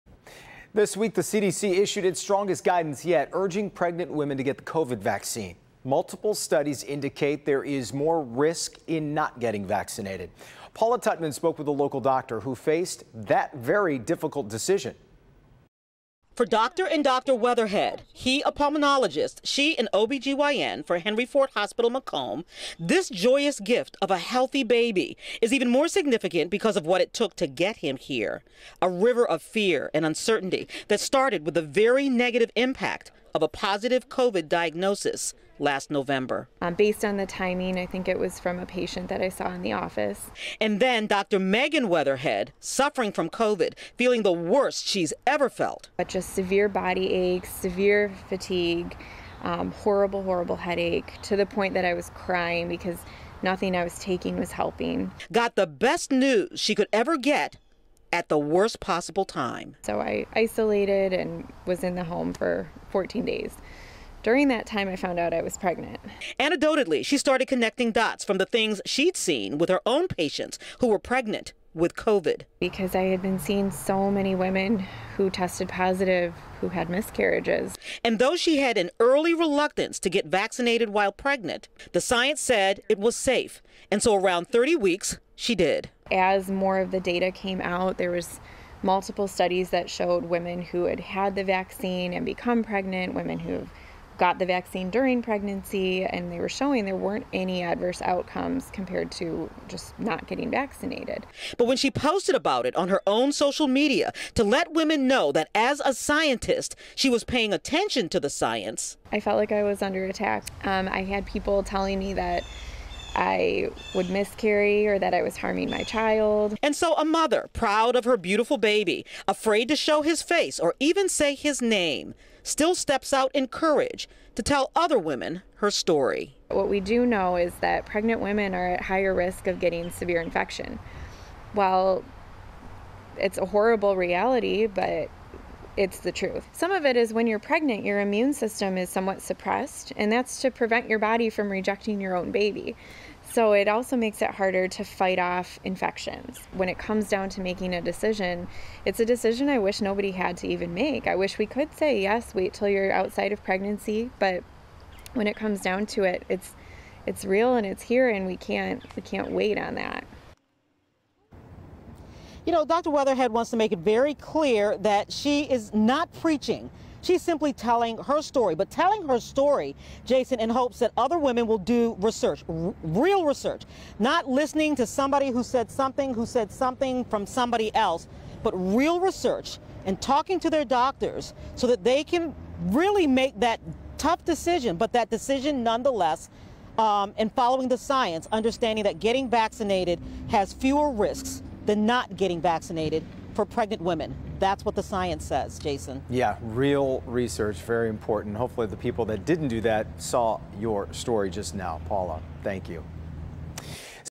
Henry Ford Health doctor receives COVID vaccine while pregnant
Scientific data continues to verifying that getting a vaccine against COVID is not only safe, but highly advised.